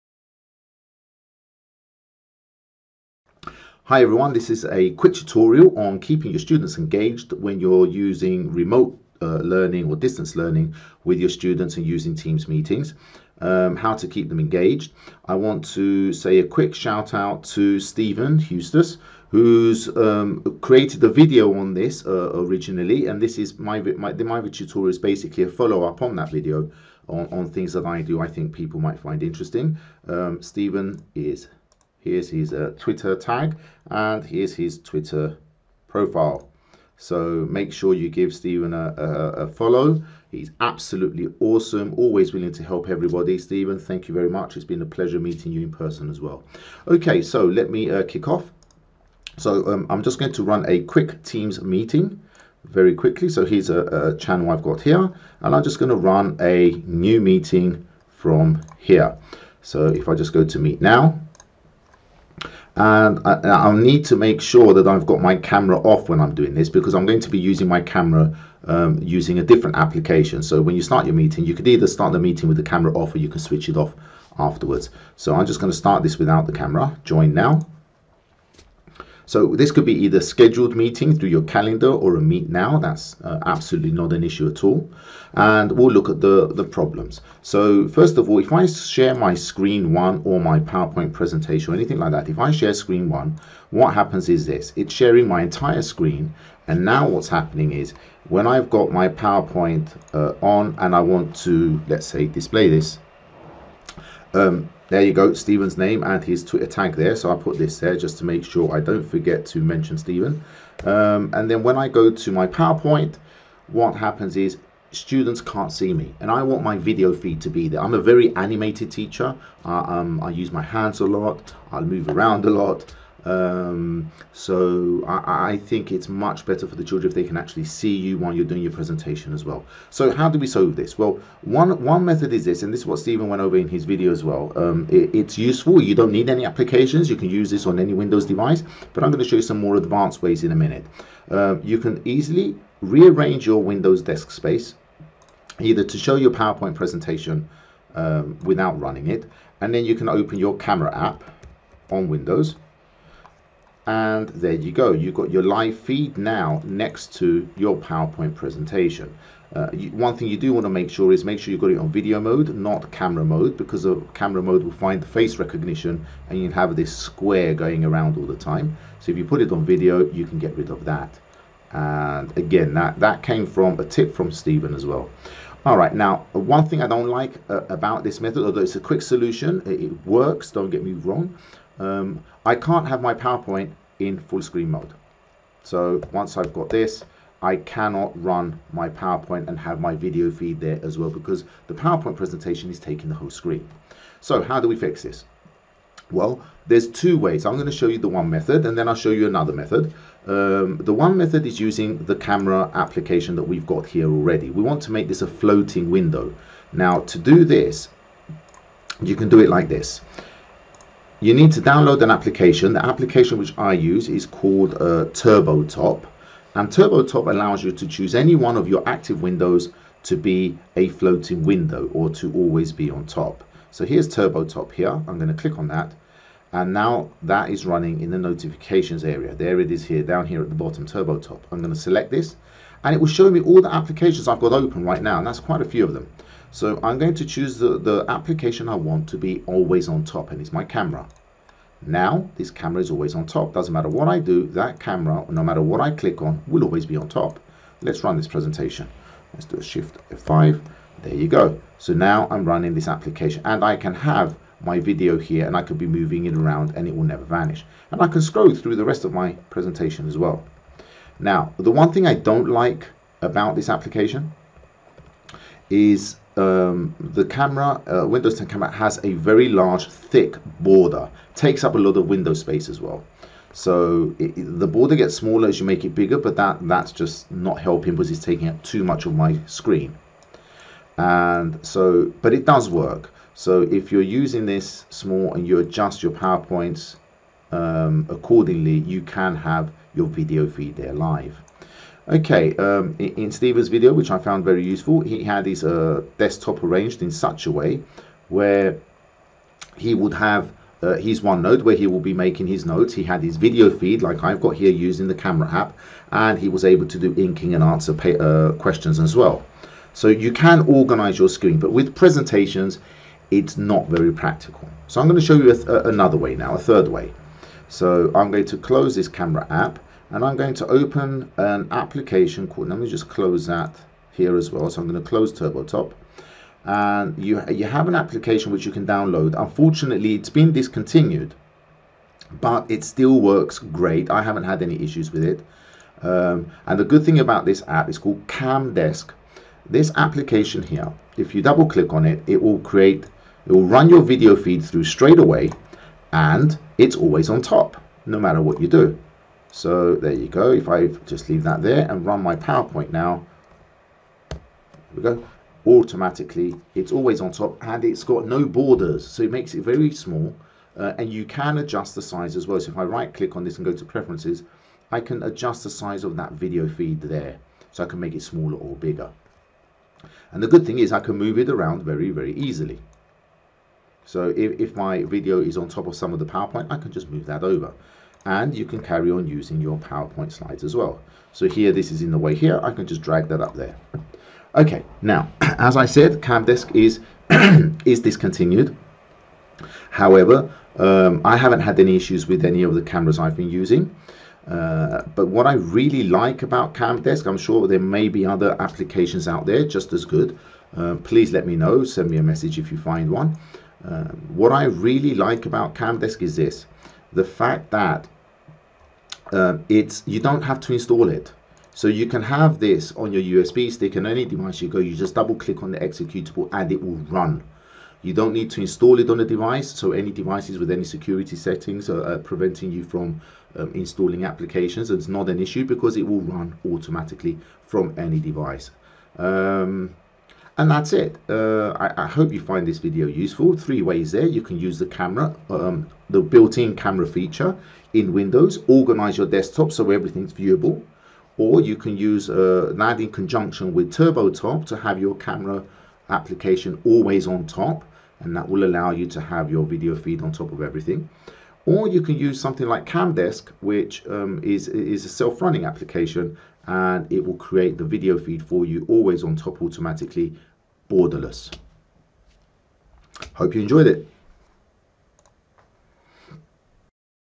Teams meetings, keeping students engaged
This video tutorial is about keeping your students engaged while you hare you screen in a Teams Meeting for distance learning by streaming your video feed while sharing your screen. A big thanks to Stephen Eustace @eustace_stephen for tips in his video